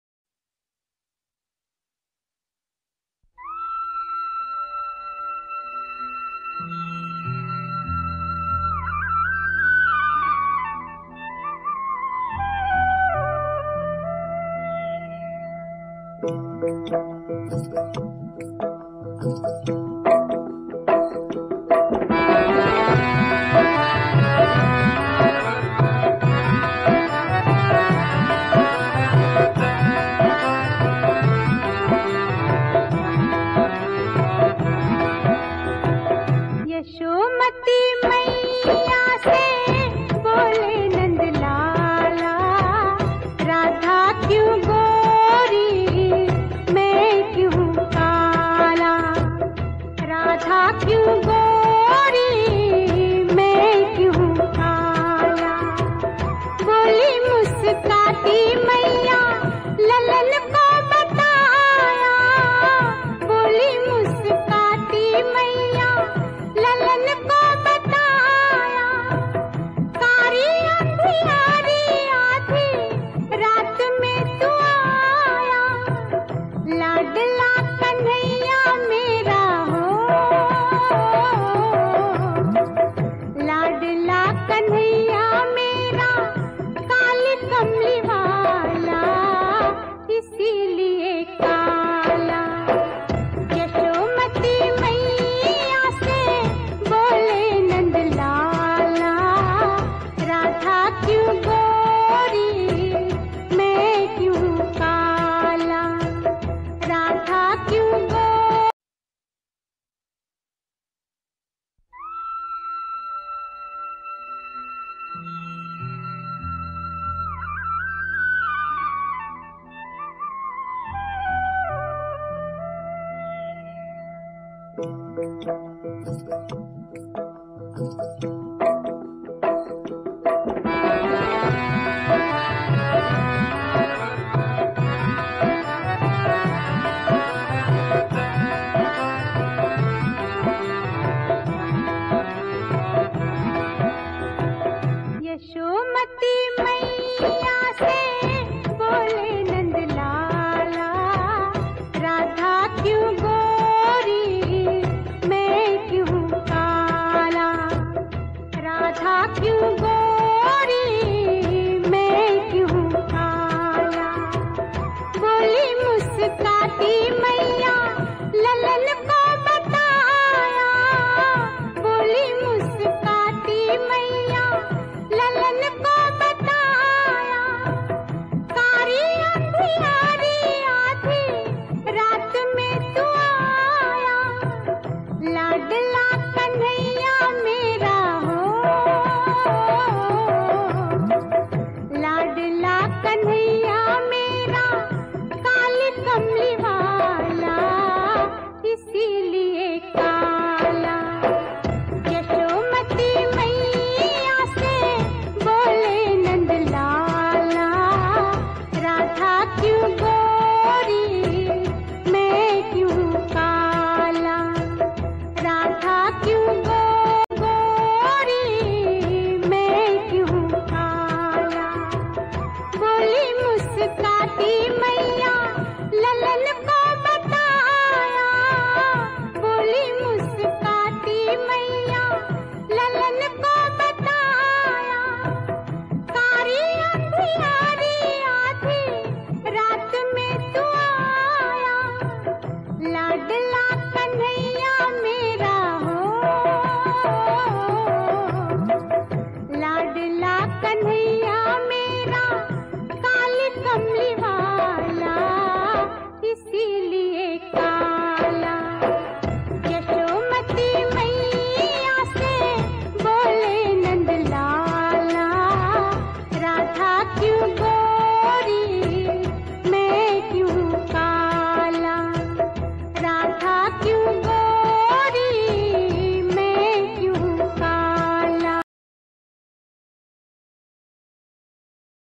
Radhe Govinda's Jhulan - Badkulla Adyapeath
Radhe Govinda's Jhulan Yatra at Badkulla Ashram . 17th August, 2013.